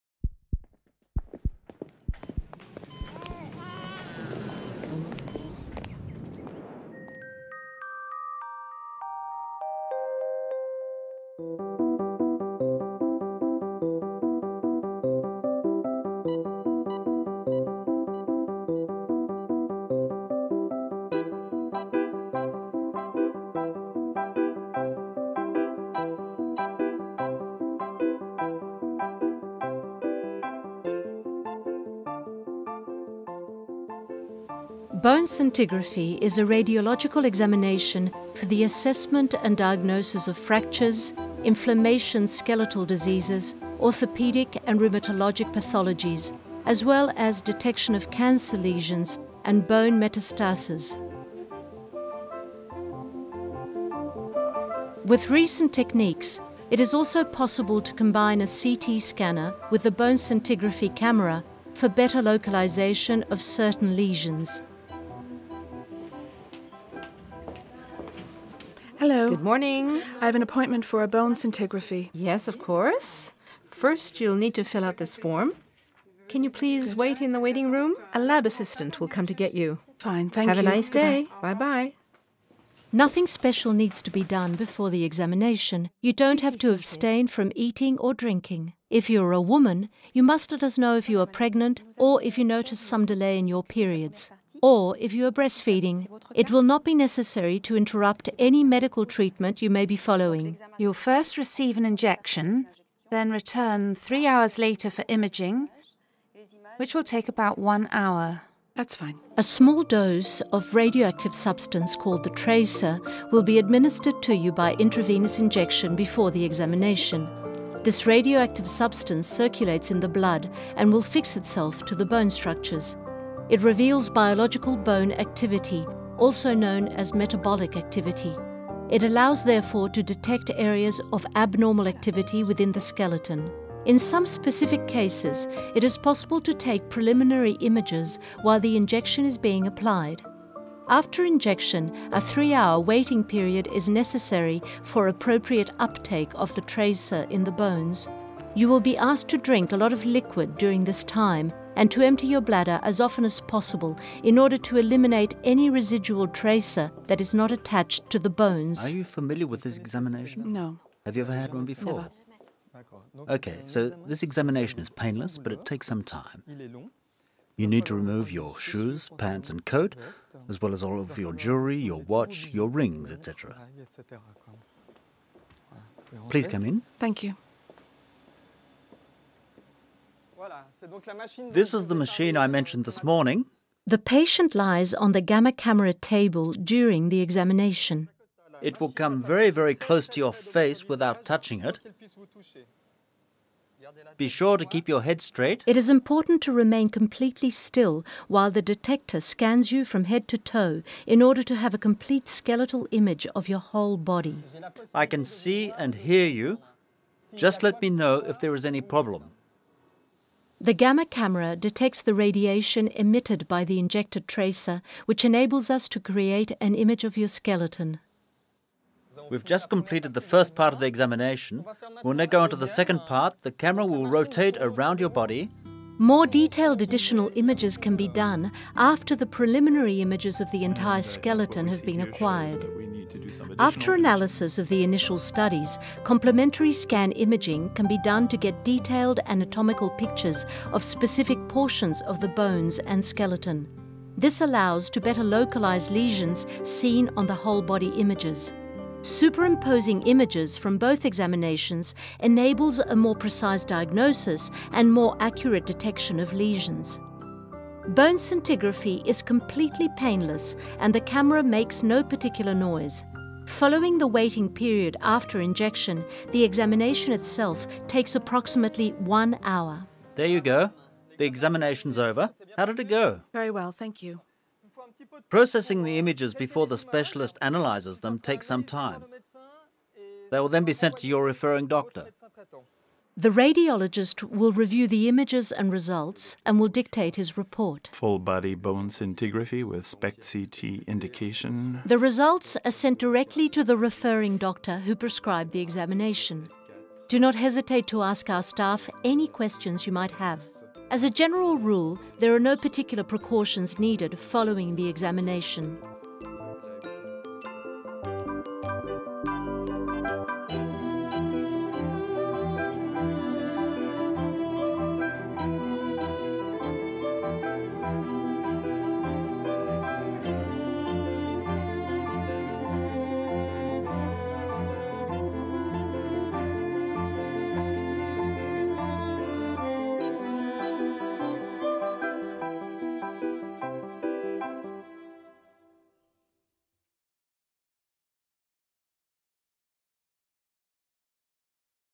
Bone Scintigraphy
Bone scintigraphy is a radiological skeletal examination that assists in diagnosing fractures, inflammatory skeletal illness, orthopedic and rheumatologic pathologies, as well as locating certain cancerous bone metastasis.

It is sometimes possible to locate certain lesions using a scanner in combination with scintigraphy.